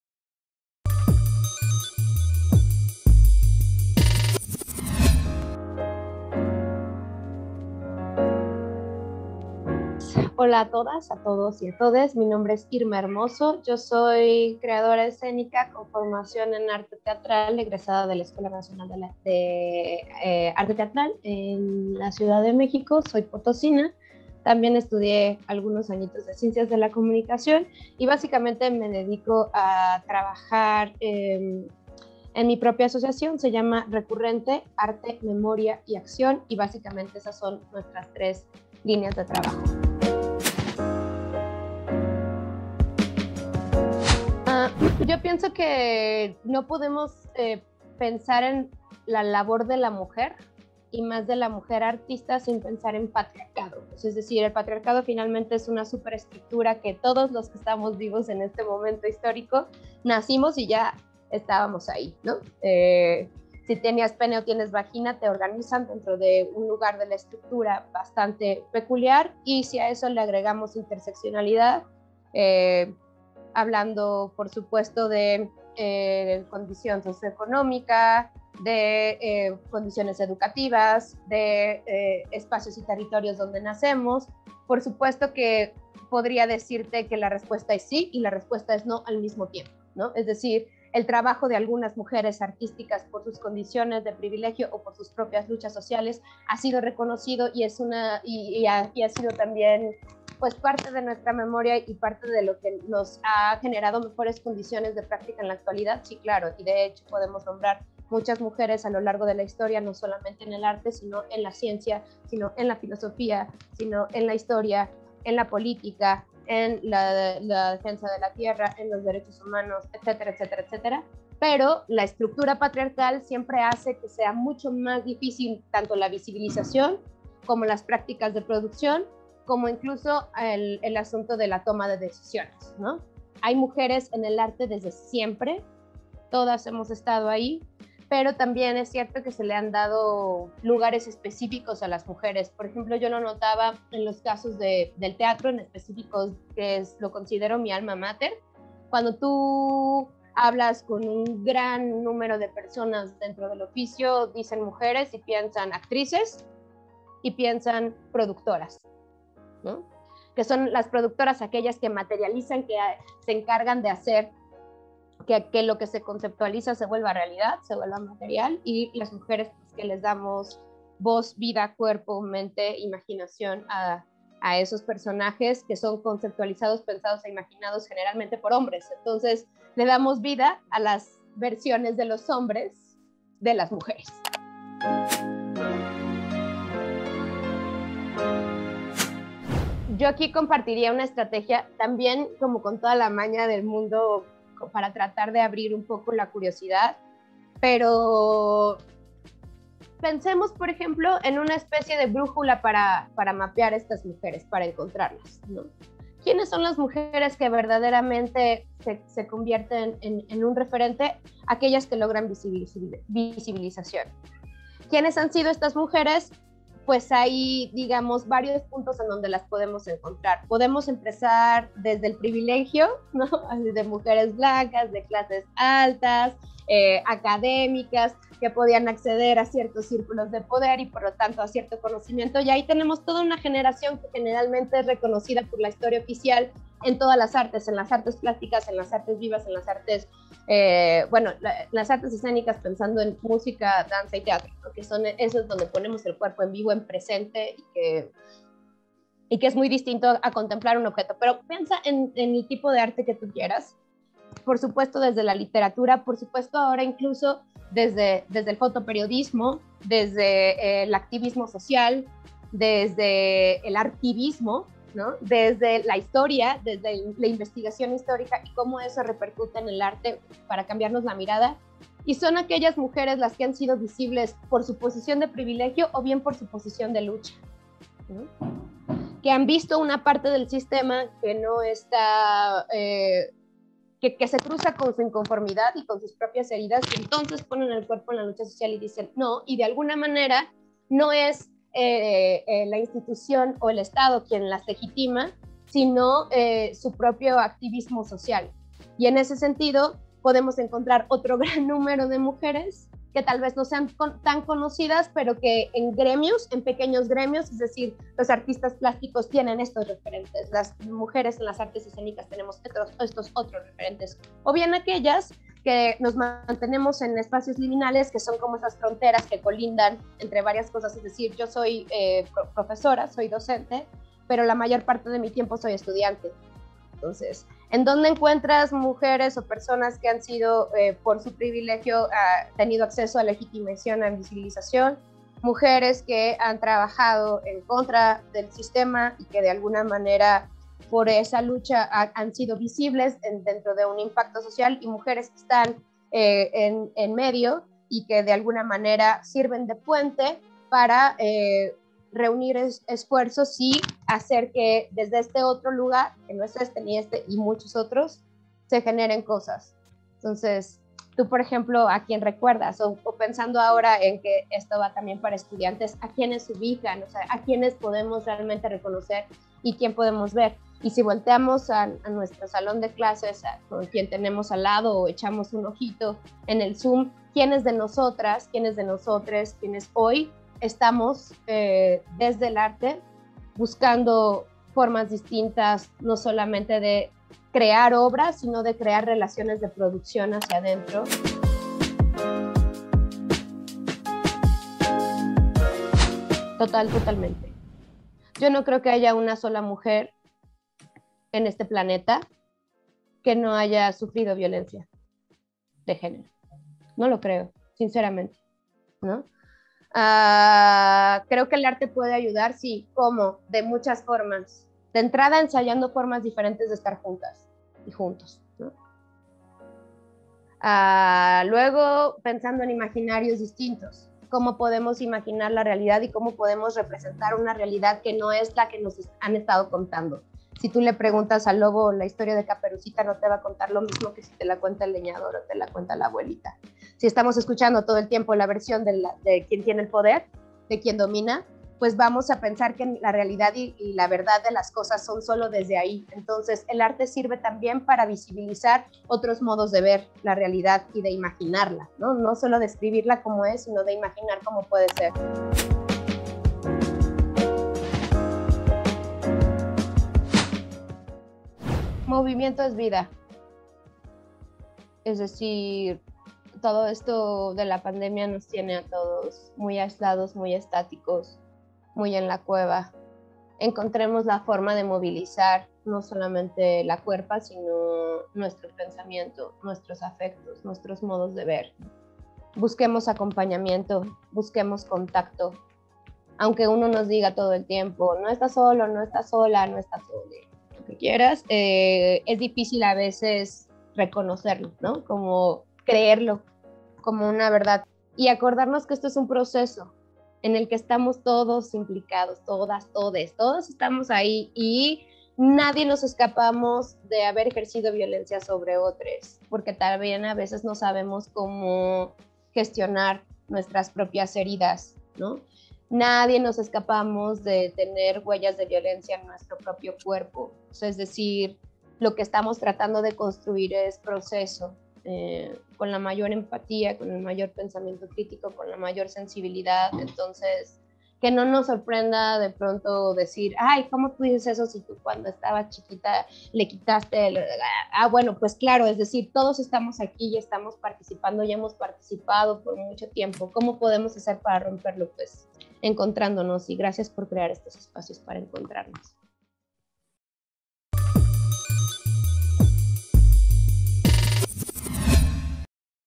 Reflexiones filosóficas en torno al arte y el espacio escénico por la Mtra. Irma Hermoso (Luna)
La Coordinación Académica en Arte en el marco de las jornadas 25N: Espacios Universitarios Libres de Violencia hacia las Mujeres presenta la capsula Reflexiones filosóficas en torno al arte y el espacio escénico por la Mtra. Irma Hermoso (Luna).